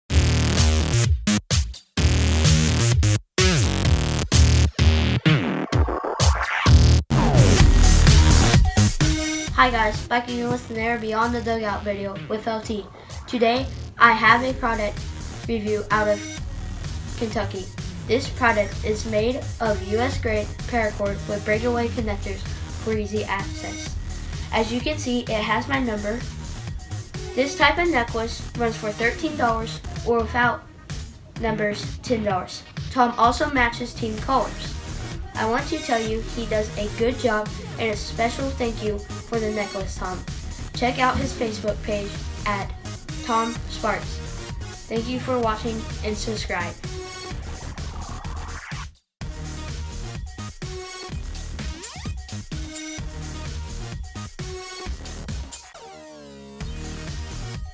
Beyond the dugout with LT: USA grade Para cord BASEBALL necklace
USA USA USA grade para cord  baseball necklace made by Tom Sparks special thanks to Tom Sparks for sending this baseball necklace if you like the video above please like and subscribe to to my channel